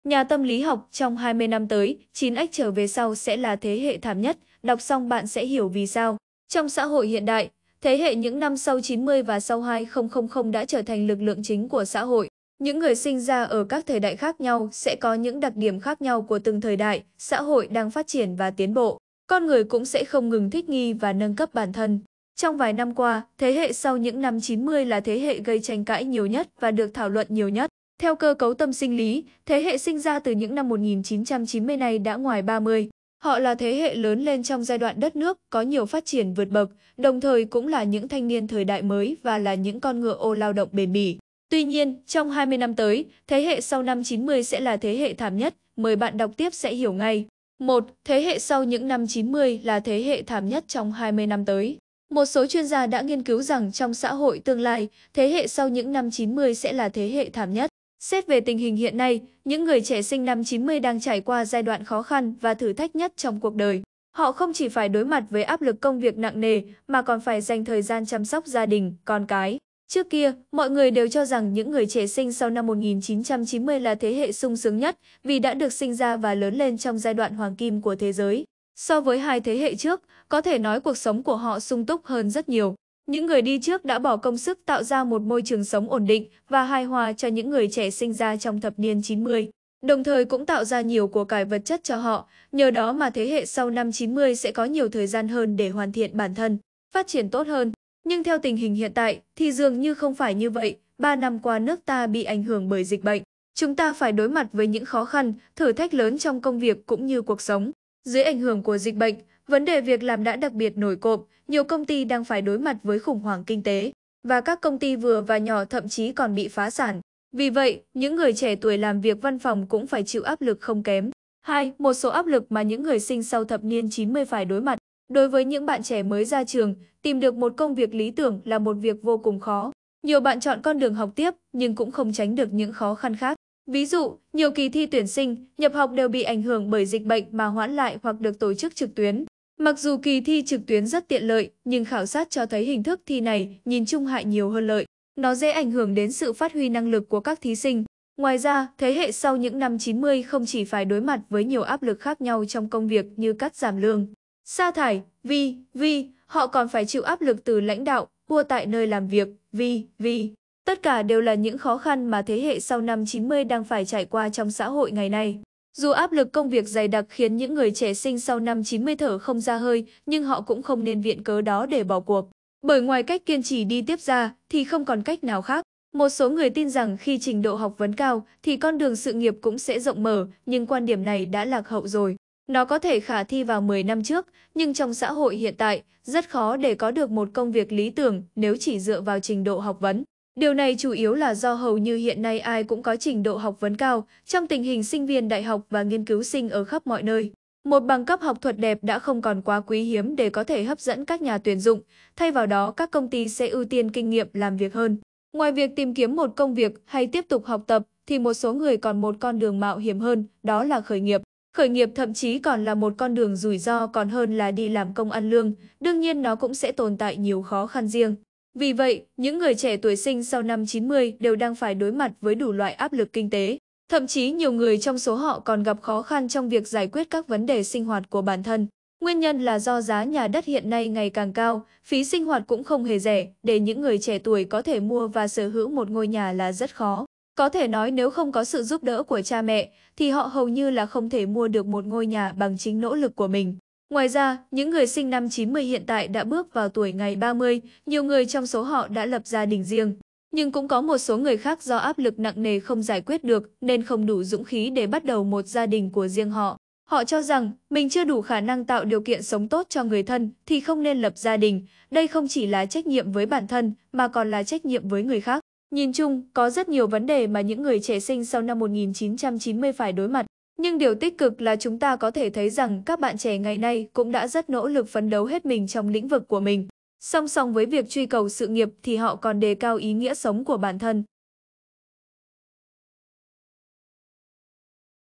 Nhà tâm lý học: "Trong 20 năm tới, 9x trở về sau sẽ là thế hệ THẢM NHẤT"
Trong xã hội hiện đại, thế hệ những năm sau 90 và sau 2000 đã trở thành lực lượng chính của xã hội. Những người sinh ra ở các thời đại khác nhau sẽ có những đặc điểm khác nhau của từng thời đại, xã hội đang phát triển và tiến bộ, con người cũng sẽ không ngừng thích nghi và nâng cấp bản thân.